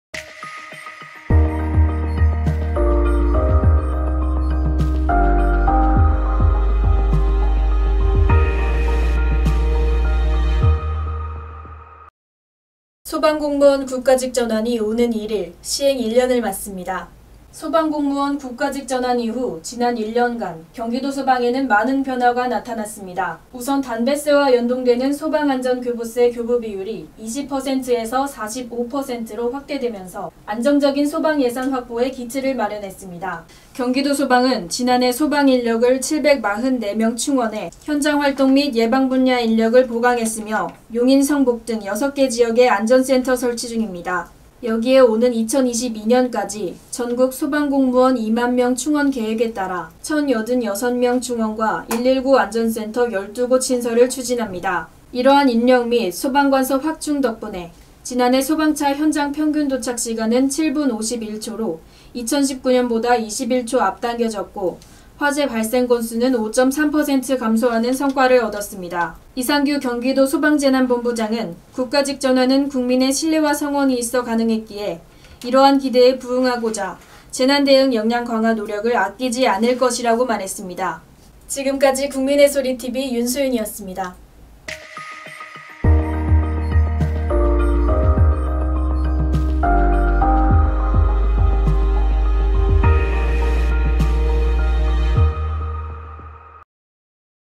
4월 1일 소방 국가직 전환 1년…경기도소방의 변화는? 국민의소리TV 경기포털TV 윤소윤기자
소방공무원 국가직 전환이 오는 1일 시행 1년을 맞는다.

소방공무원 국가직 전환 이후 지난 1년간 경기도 소방에는 많은 변화가 나타났다. 우선, 담뱃세와 연동되는 소방안전교부세 교부 비율이 20%에서 45%로 확대되면서 안정적인 시·도 소방예산 확보의 기틀을 마련했다.

경기도소방은 지난해 소방인력을 744명 충원해 현장활동 및 예방분야 인력을 보강했으며, 용인 성복 등 6개 지역에 안전센터를 설치 중이다. 여기에 오는 2022년까지 전국 소방공무원 2만 명 충원 계획에 따라 1,086명 충원과 119안전센터 12곳 신설을 추진한다.

이러한 인력 및 소방관서 확충 덕분에 지난해 소방차 현장 평균 도착 시간은 7분51초로 2019년보다 21초 앞당겨졌고, 화재발생 건수는 5.3% 감소하는 성과를 얻었다.

이상규 경기도 소방재난본부장은 “국가직 전환은 국민의 신뢰와 성원이 있어 가능했기에 이러한 기대에 부응하고자 재난대응 역량 강화 노력을 아끼지 않을 것”이라고 말했다.